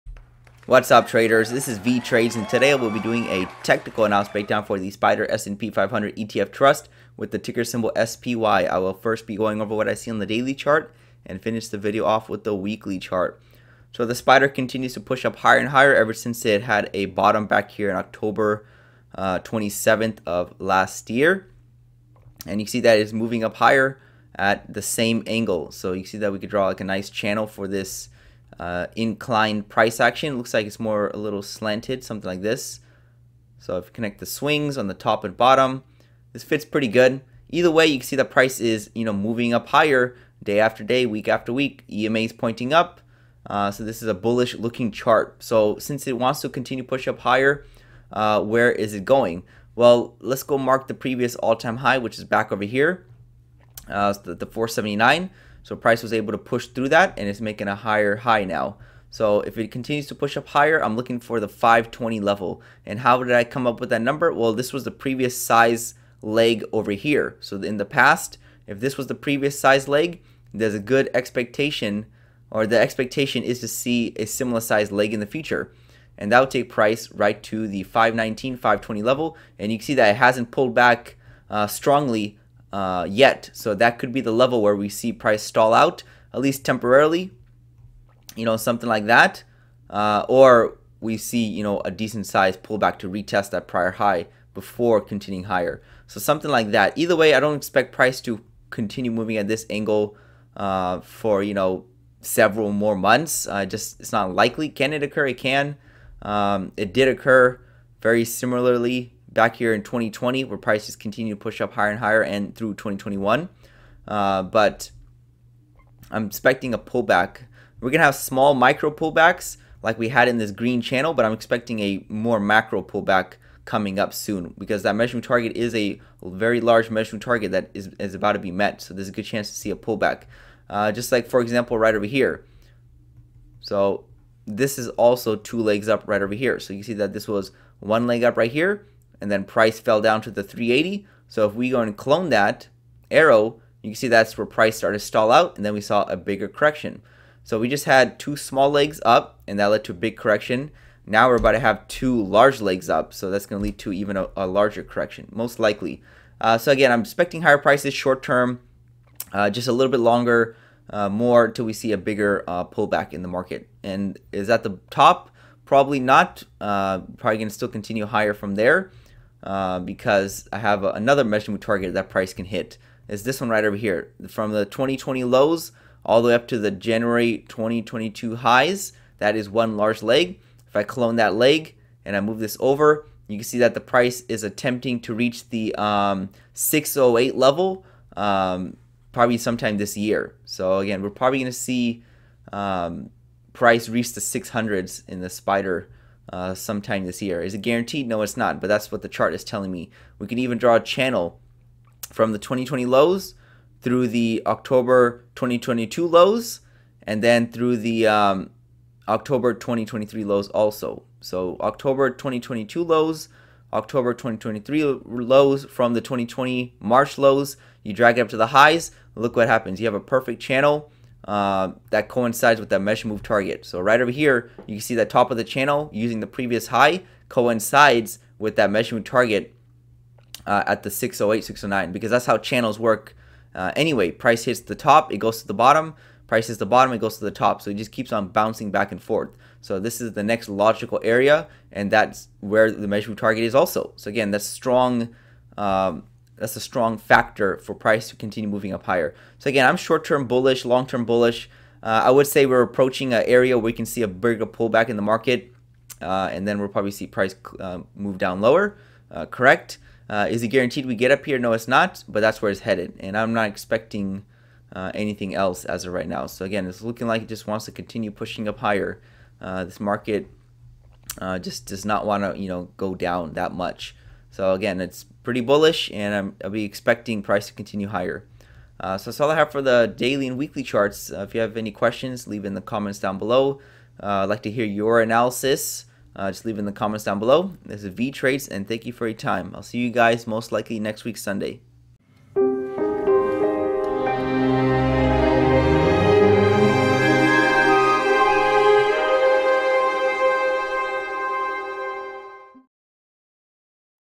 SPY ETF Analysis
Hello YouTube❗,

This is video number 1,222 on the channel. I will be doing a Technical Analysis breakdown for the SPY with price targets.

❗ Vee Trades Technical Analysis Membership is now live. ❗